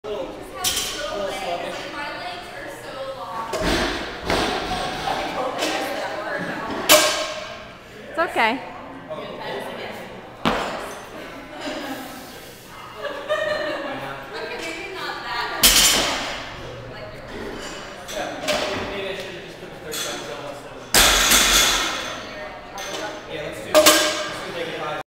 Sarah Cleans Part 2
She still hasn't settled her mind and makes similar errors as in part one.